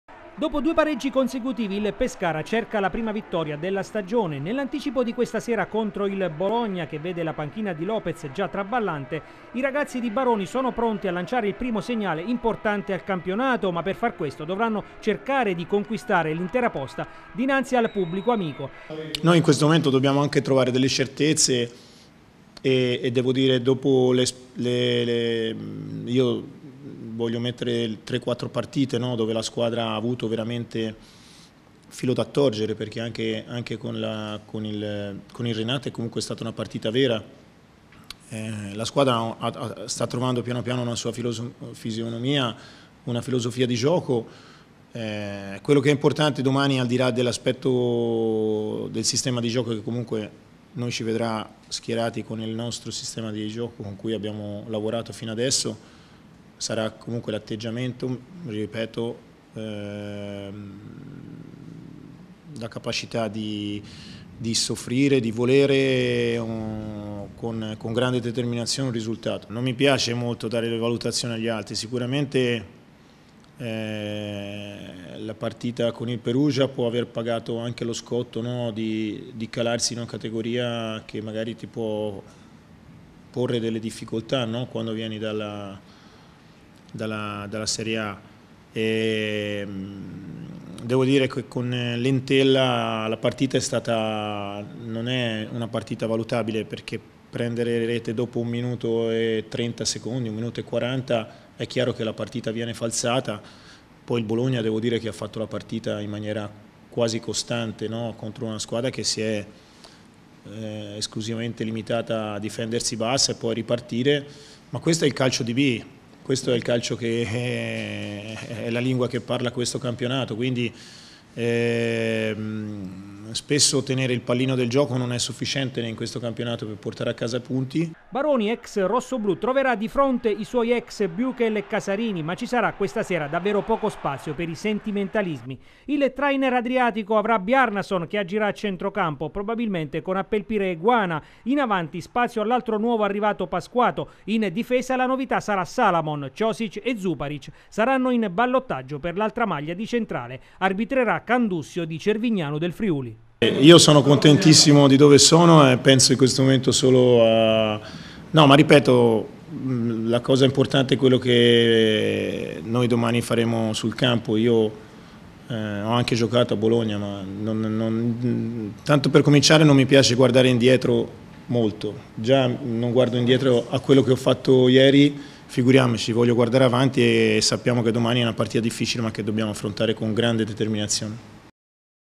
Pescara, Baroni pre partita
Anticipo di lusso, questa sera allo Stadio Adriatico Cornacchia, dove arriva il Bologna. Baroni è pronto a cambiare formazione ma manterrà lo stesso modulo di gioco. Dal primo minuto giocheranno Salamon e Guana. Pescara-Bologna sarà diretta da Candussio di Cervignano del Friuli